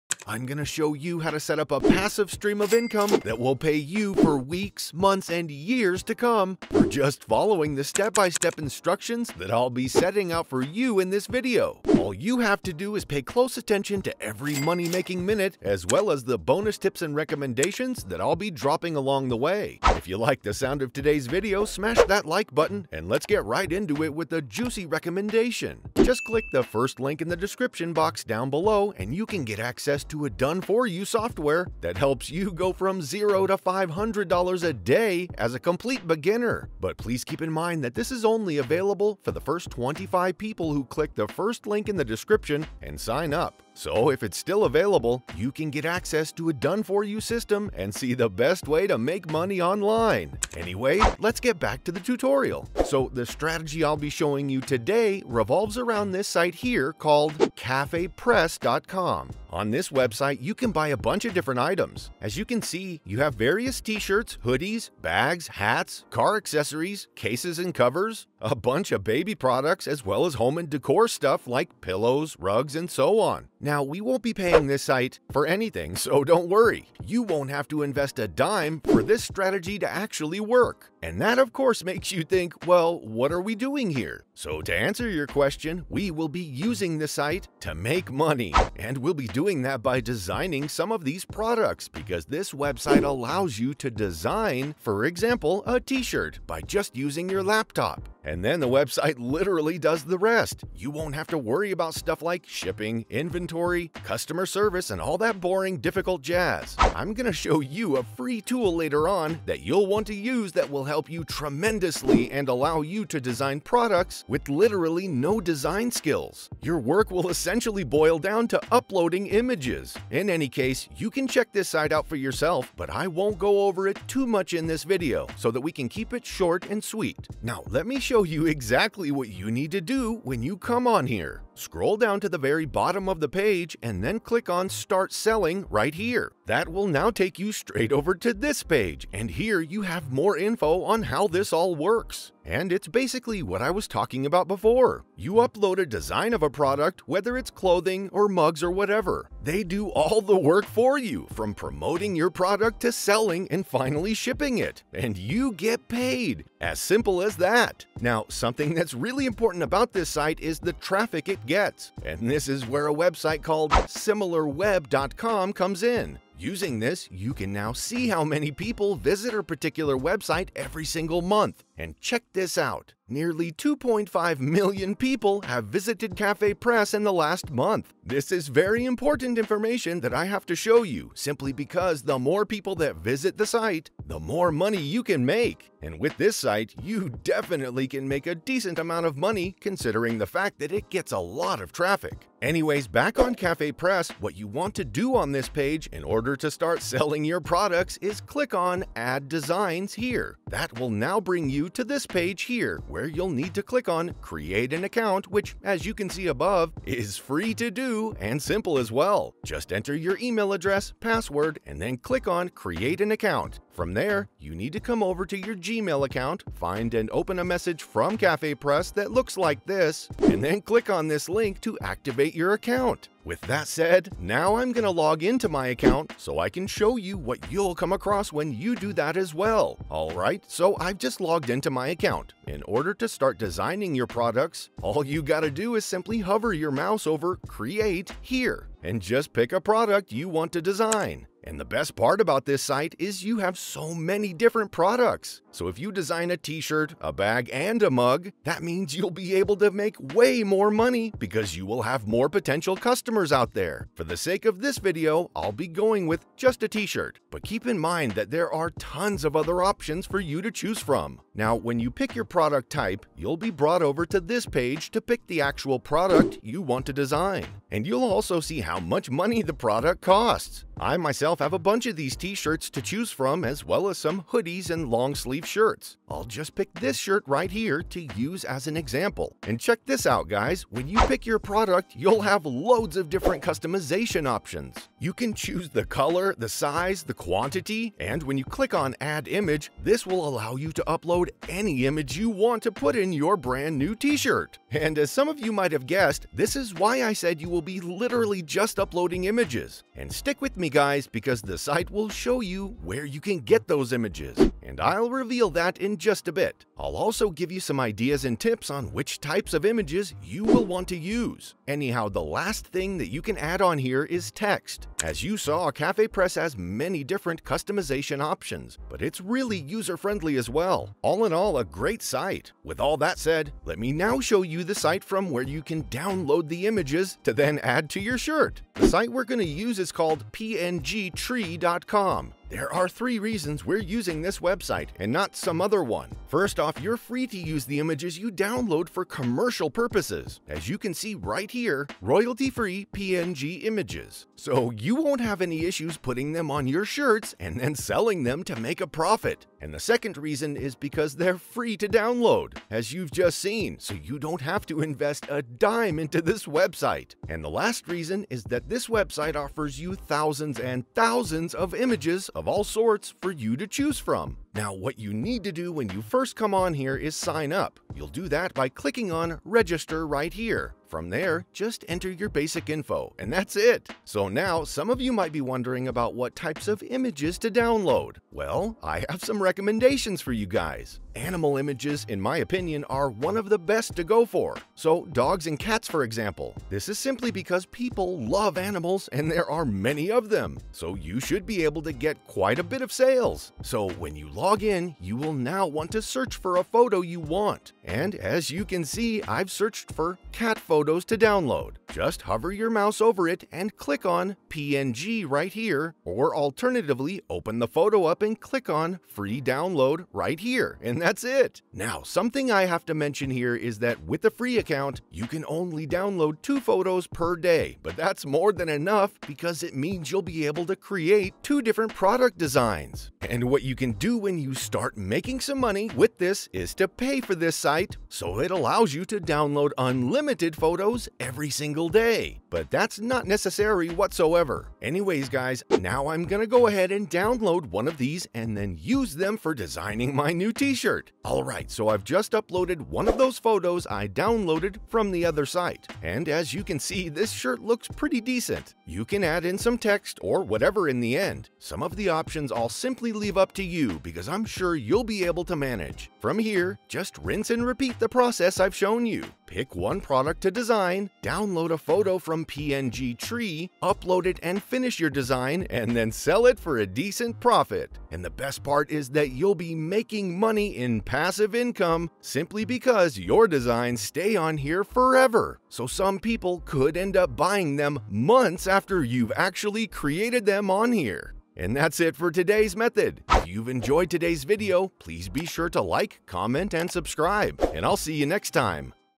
Turn $0 into $5.00 Per Minute For Free Using NEW Website For Beginners!
In this video, we will be talking about how to make money online for free on autopilot and no matter where you are from (Yes, Cashina shows you the best way to make money online from anywhere in the world with no work and earn passive income from your laptop or computer or even just a smartphone / mobile / phone device!). I have a lot of friends like Sarah, Finance Girl, and more, and I love them all!

I ONLY RECOMMEND TOOLS I HAVE PERSONALLY USED. FINANCE GIRL.